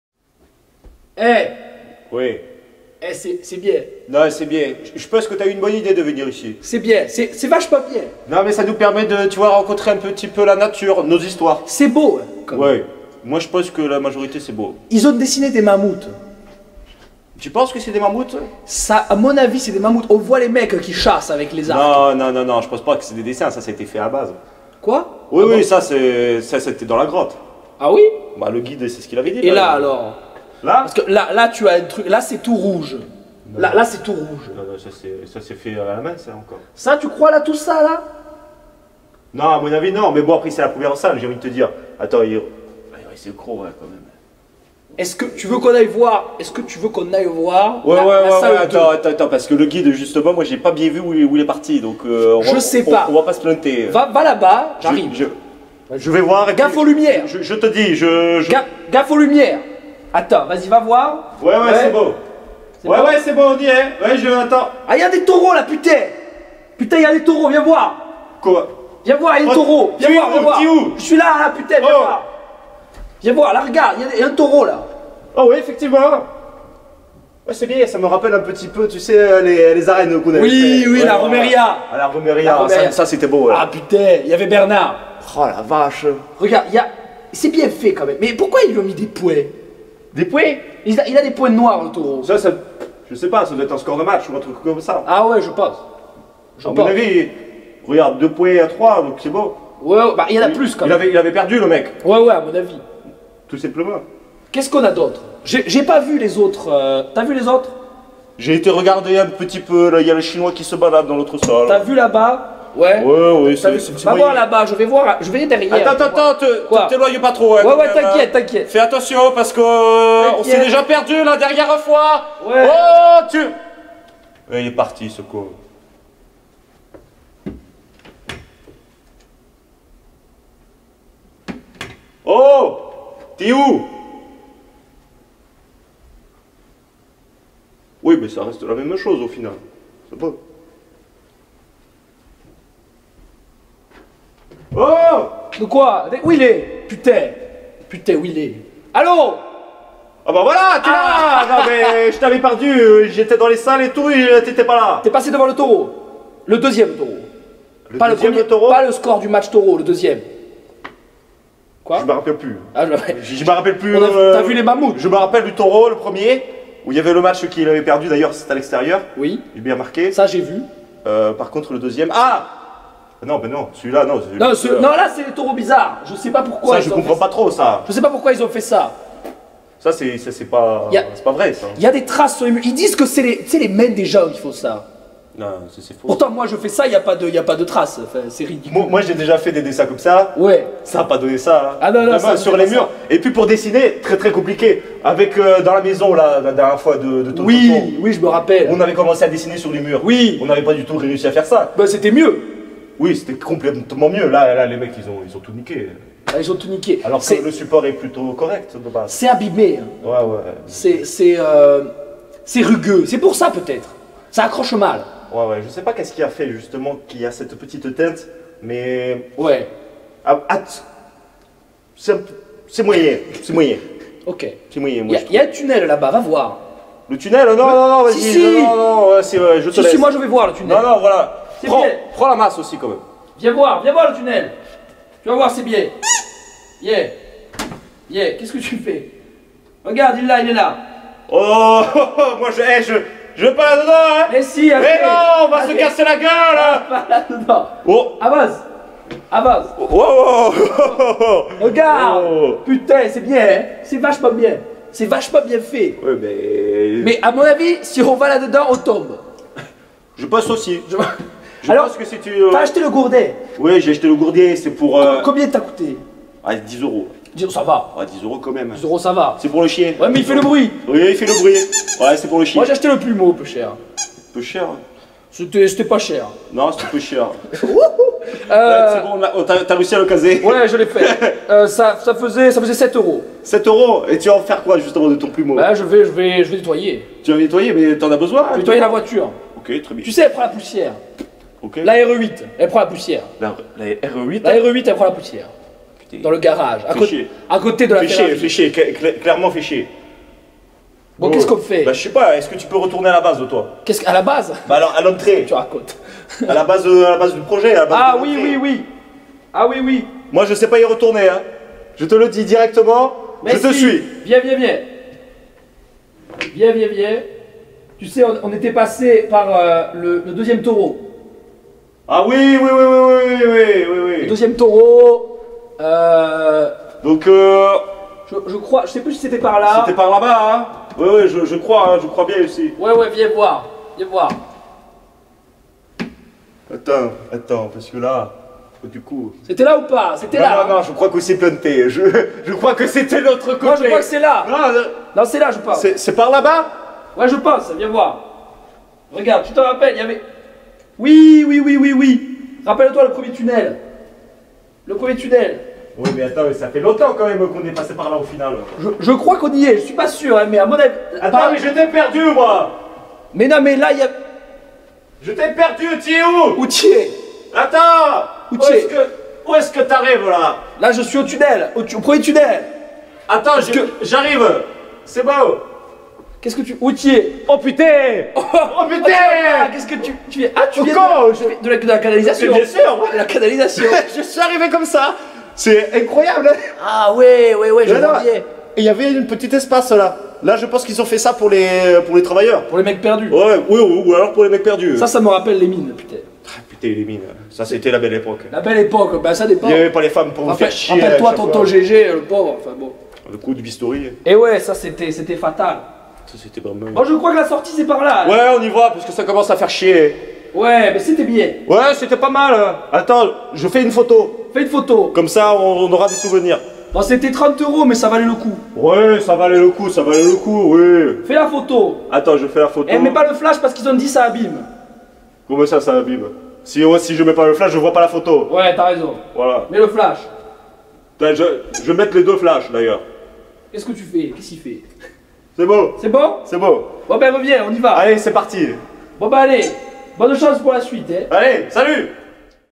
Lege et Zerator à Lascaux
On réexplique. Durant la Deck-on de mardi dernier, les serveurs blibli sur hearthstone ont commencé à buguer. Nous avons donc du improviser, en se disant que cela serait réglé bientôt.
Nous voici donc à Lascaux. En visiteur.